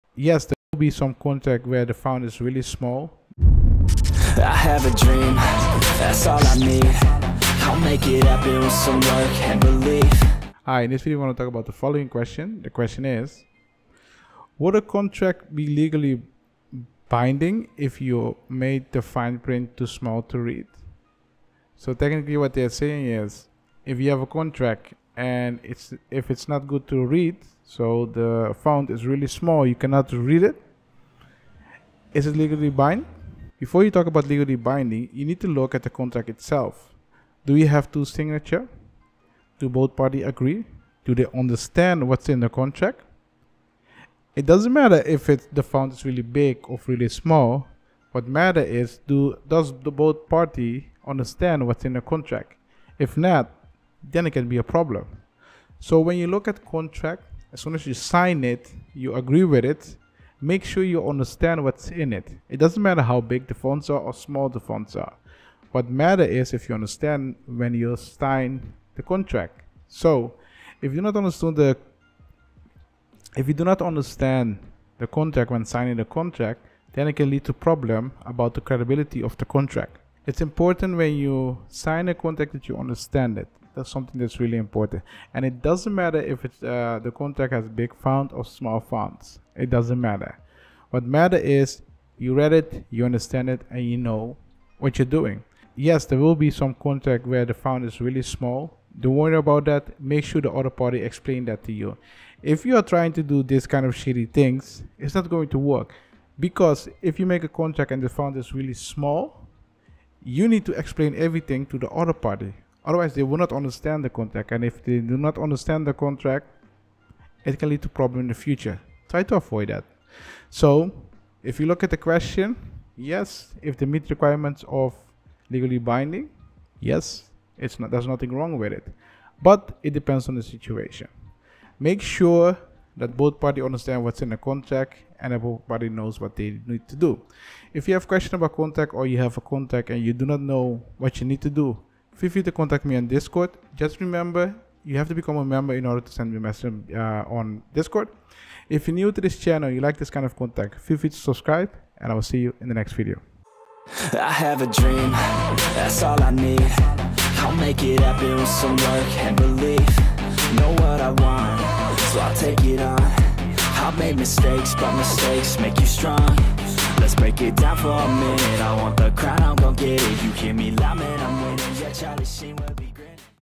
Would a Contract Be Legally Binding If You Made the Fine Print Too Small To Read? | Martisz Legal
In today's video, we're discussing the following question: "Would a Contract Be Legally Binding If You Made the Fine Print Too Small To Read?" This video will be very helpful for people that want to learn more about contracts.